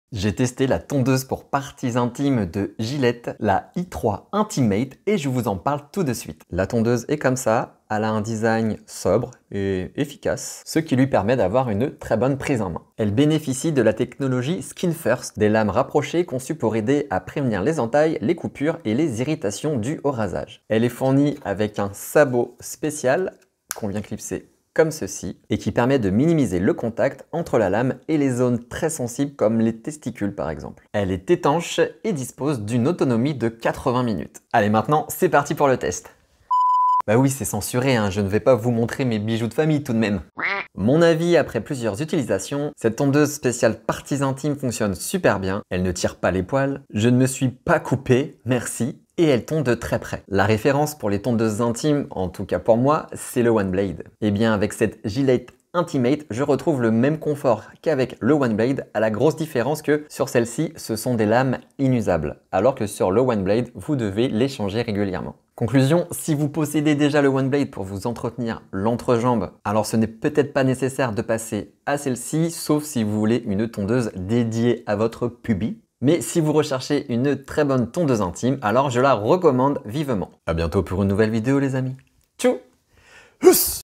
Je teste la tondeuse intime GILLETTE INTIMATE !
Découvrez mon test de la nouvelle Gillette Intimate i3 et mon avis !

• Certains produits présentés peuvent être des produits reçus gratuitement, cela ne change en rien mes avis sur ces derniers.